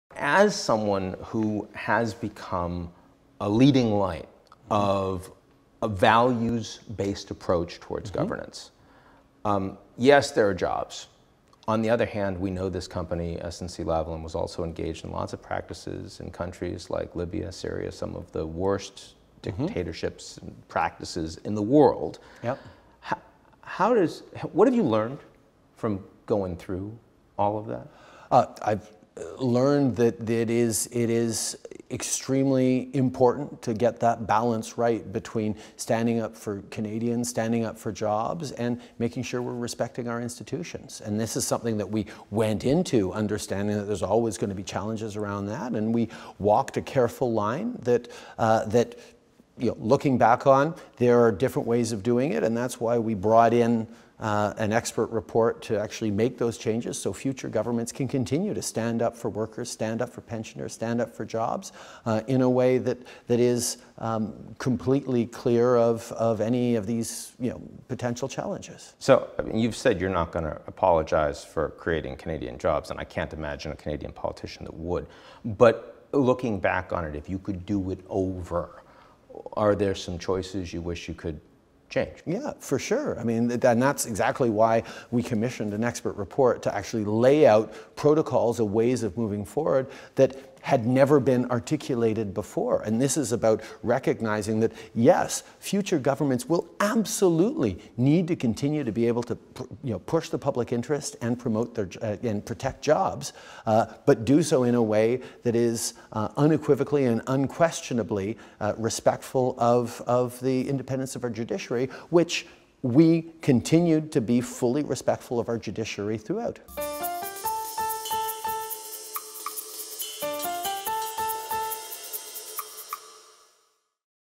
What Justin Trudeau Learned from the SNC–Lavalin Scandal
Justin Trudeau: "I learned that it is extremely important to get that balance right between standing up for Canadians, standing up for jobs, and making sure we're respecting our institutions."